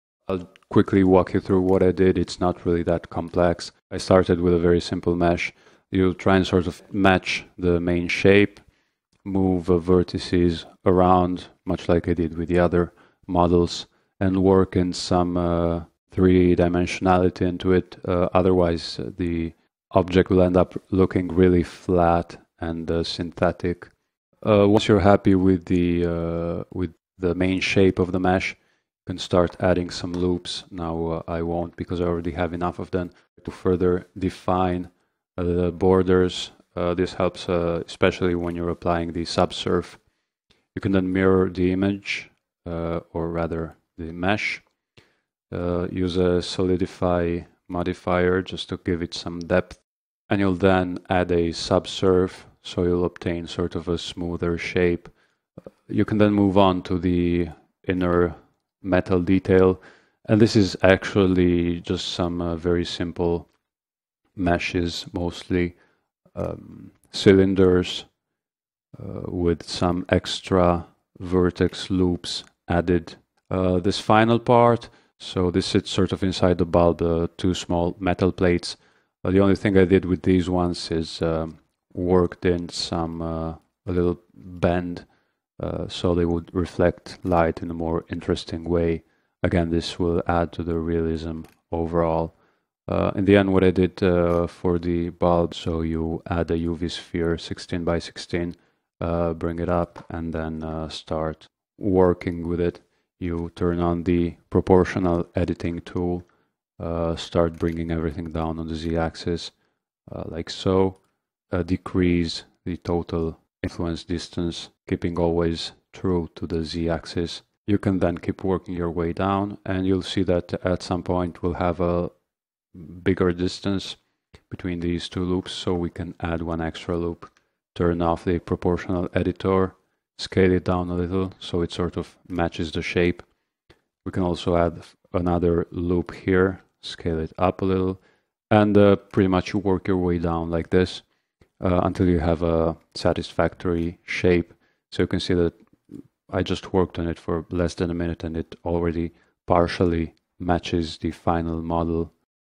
Blender Tutorial: Modeling Small Details Makes The Difference - Blender vs. The Light Bulb Part 4
In this video, I'll show you how to quickly model small parts that can add to the overall realism of a 3D model and create a sense of complexity with little effort,